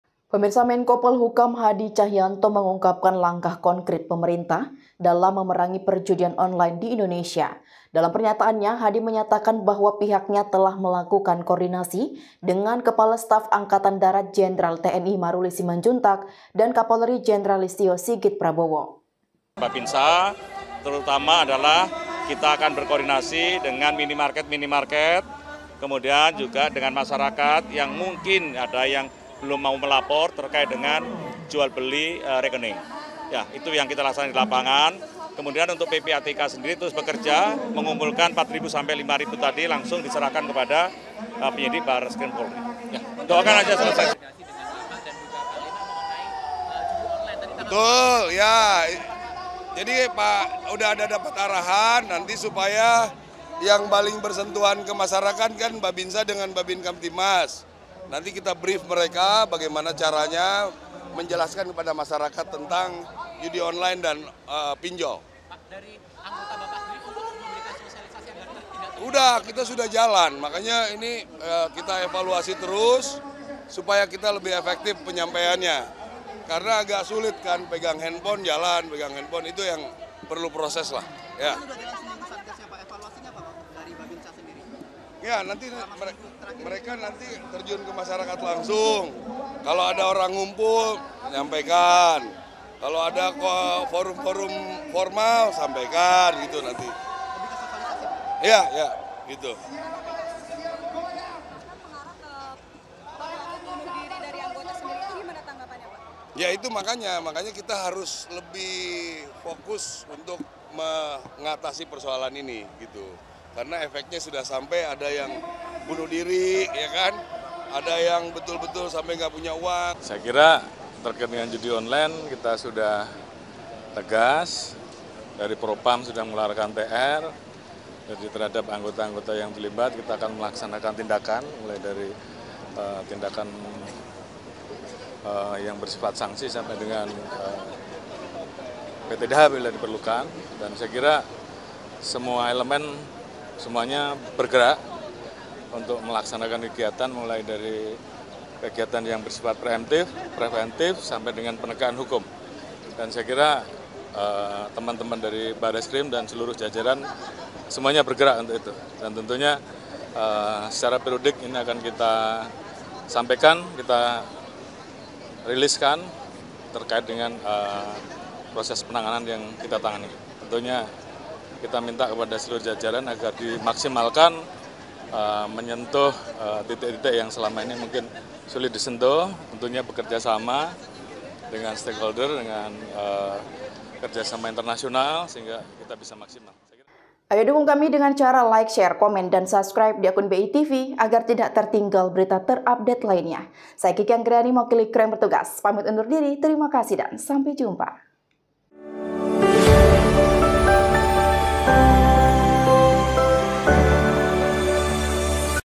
Upaya Menko Polhukam dan Jajaran TNI-Polri dalam Memberantas Judi Online
Menko Politik, Hukum, dan Keamanan (Menko Polhukam) Hadi Tjahjanto mengungkap langkah konkret pemerintah dalam memerangi perjudian online di Indonesia. Dalam pernyataannya di Jakarta pada Sabtu (22/6/2024), Hadi menyatakan bahwa pihaknya telah melakukan koordinasi dengan Kepala Staf Angkatan Darat (KSAD) Jenderal TNI Maruli Simanjuntak dan Kapolri Jenderal Listyo Sigit Prabowo